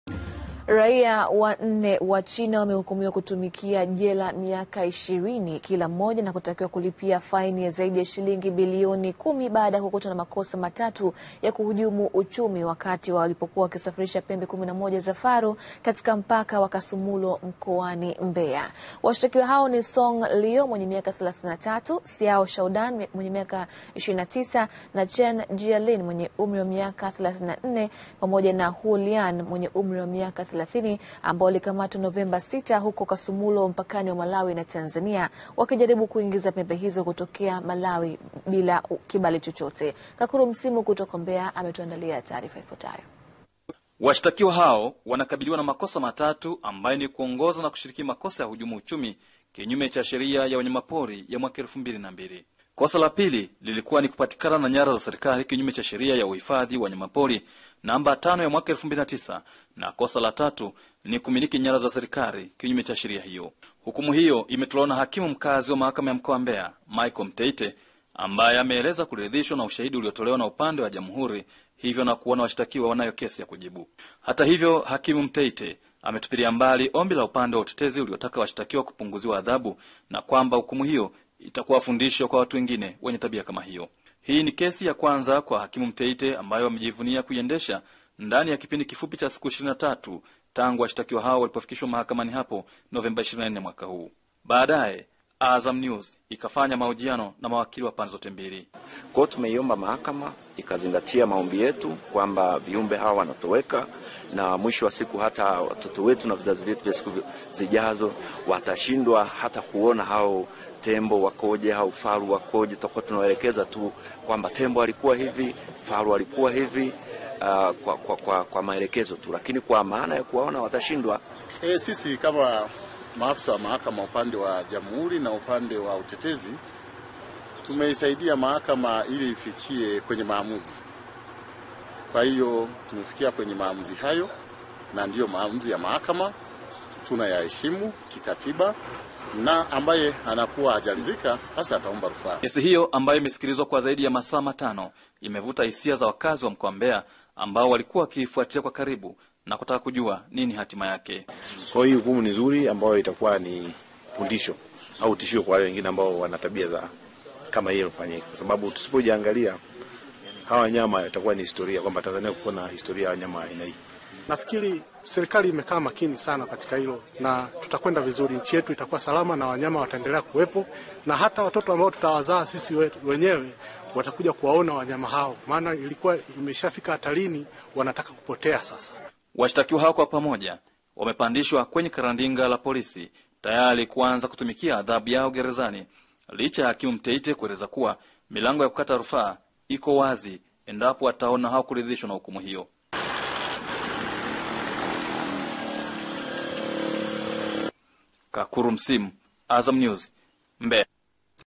Wachina 4 Mbaroni Kwa Kuhujumu Uchumi Wa Nchi
Raia wa 4 wa China wamehukumiwa kutumikia Jela miaka 20 na faini ya shilingi Bilioni 10 kwa tuhuma za kuhujumu uchumi wa nchi.